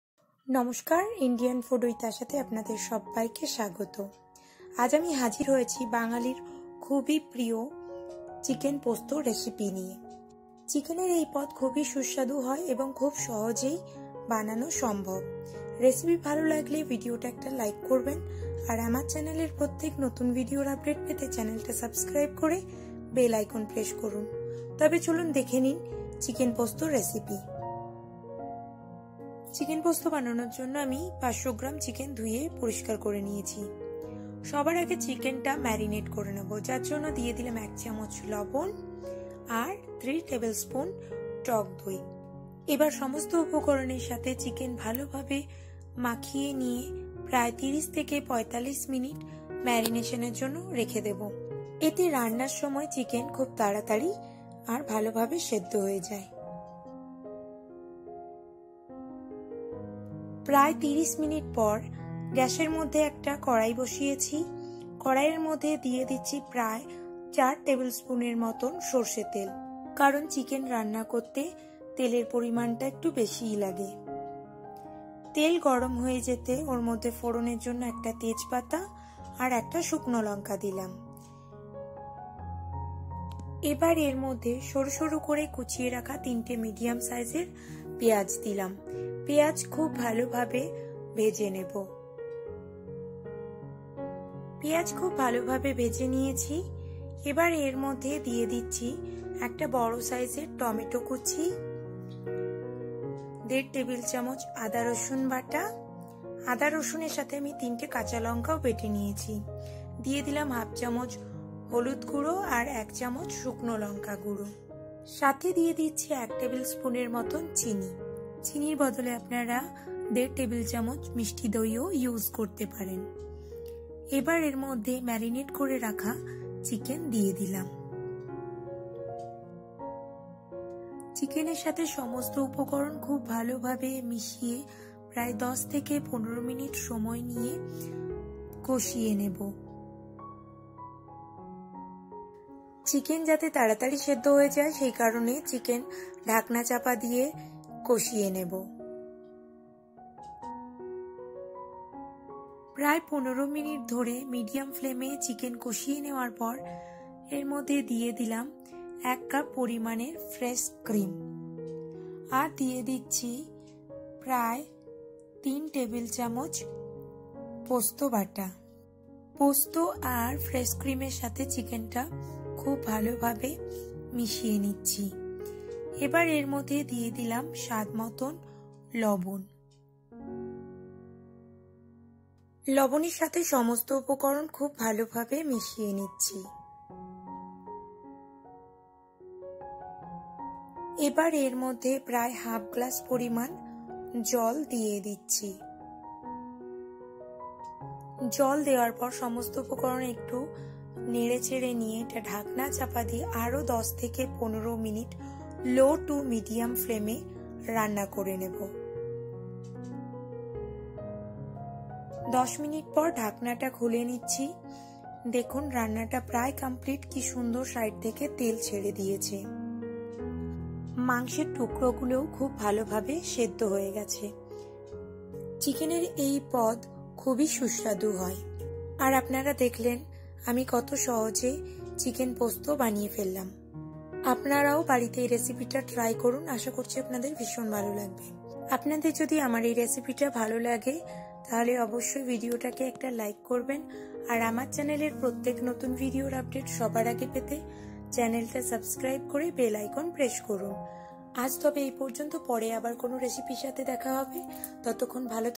আজই বানিয়ে ফেলুন জিভে জল আনার মতো chicken posto recipe | chickenposto | bengali chicken recipe
চিকেন পোস্ত রেসিপি | chicken posto | chicken posto recipe in bengali | chicken posto recipe | bengali chicken recipe

Indian Food With Asha

আজ আমি আপনাদের সাথে chicken posto recipe সেয়ার করছি। এইপদ খুবই সুস্বাদু হয়।

ingredients :

chicken : 500 gm
posto : 3 table spoon
sorse tel :4 table spoon
piyaz : 3 ta
holud guro : 1 tea spoon
lonka guro : 1 tea spoon
chini : 1 tea spoon
tok doi: 3 table spoon
fresh cream : 1 cup